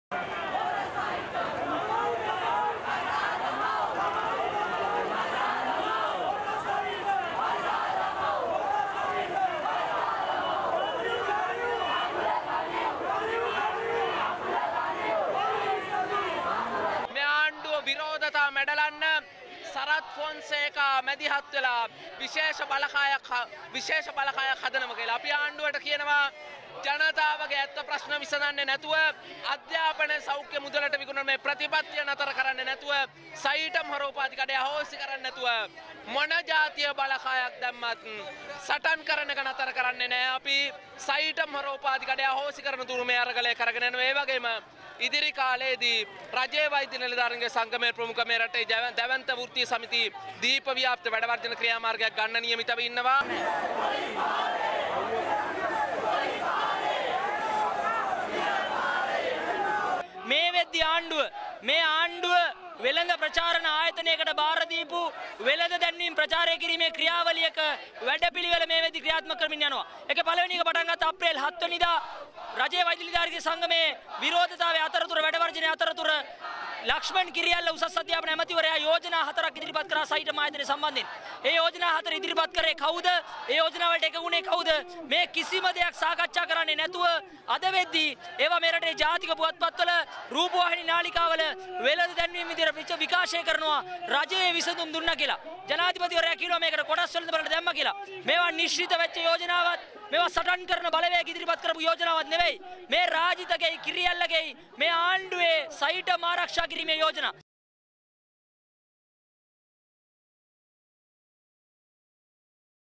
Student leaders challenge moves to set up new unit with Sarath Fonseka
ශ්‍රී ලංකාවේ විරෝධතා සහ උද්ඝෝෂණ මැඩලීම සදහා  හිටපු හමුදාපතිවරයාගේ නායකනත්වයෙන් රජය විසින් පිහිටුවීමට යන නව ආයතනයට සයිටම් විරෝධි  ශිෂ්‍ය නායකයෝ අභියෝග කර සිටිති.